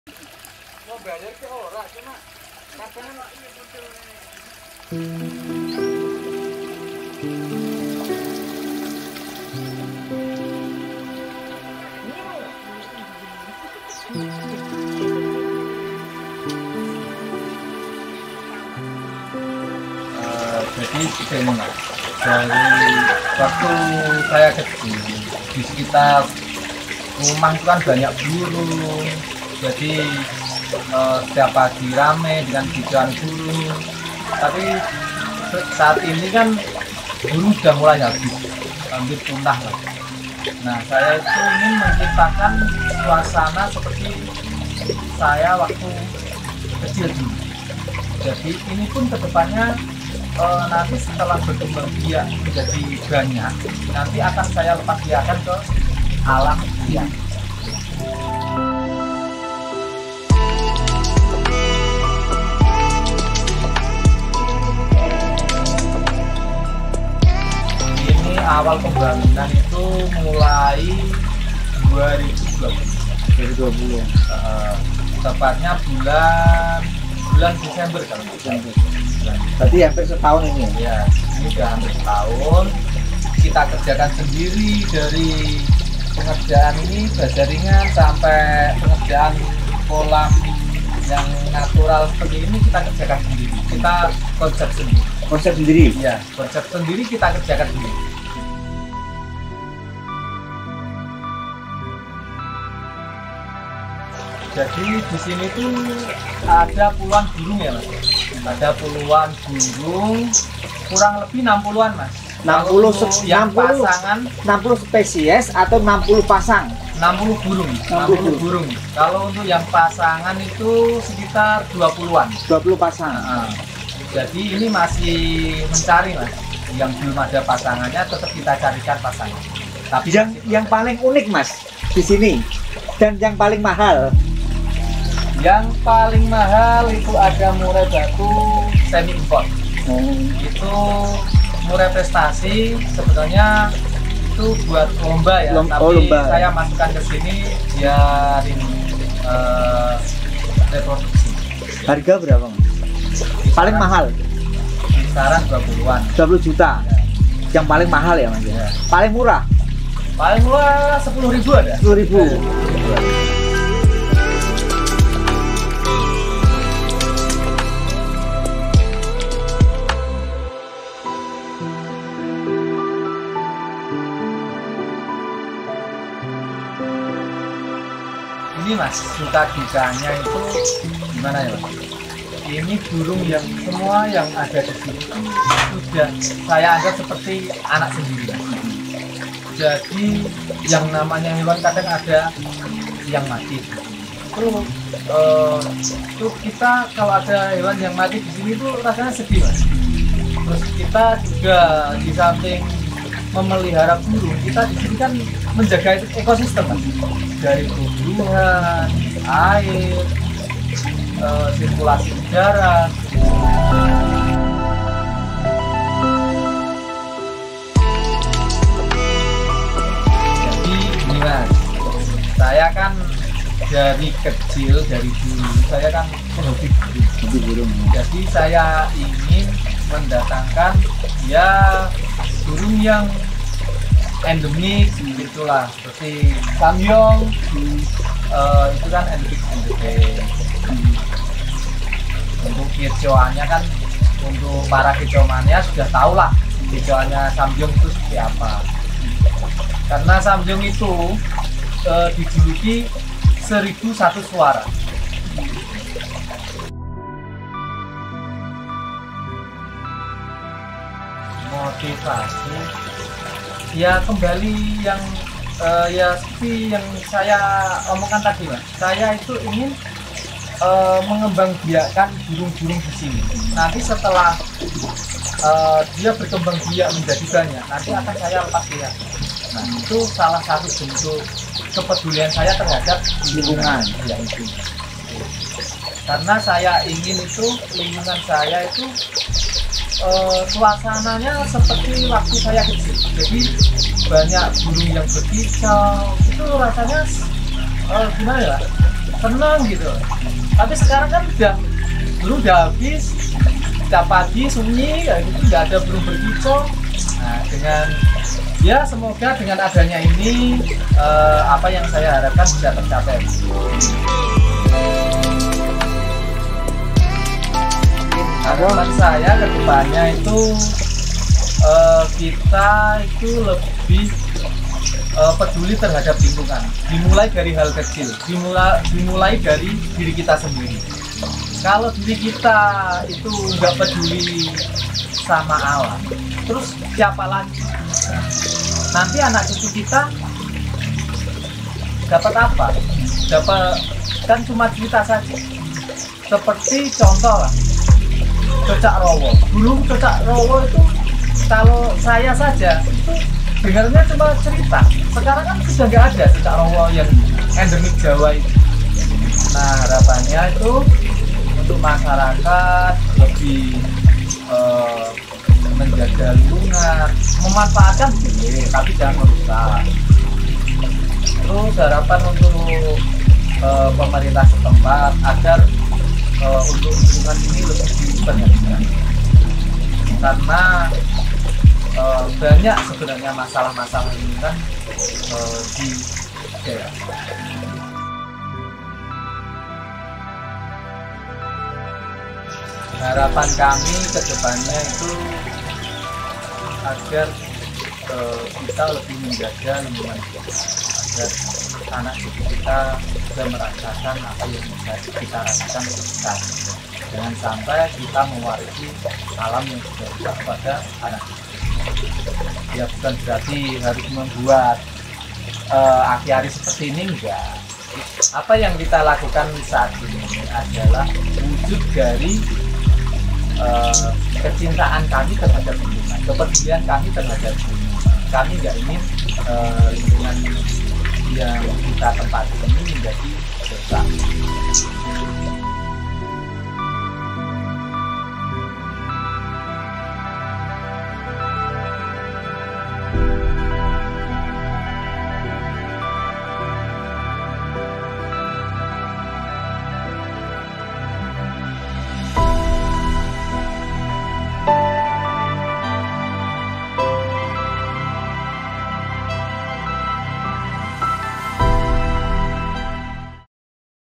INGIN SELAMATKAN HABITAT BURUNG, WARGA KEDUMULYO PATI BUAT TAMAN BURUNG
Taman burung mini minimalis di belakang rumah yang nyaman dan asri. Dengan kreatifitas, halaman kosong di belakang rumah bisa di sulap jadi tempat yang menyenangkan!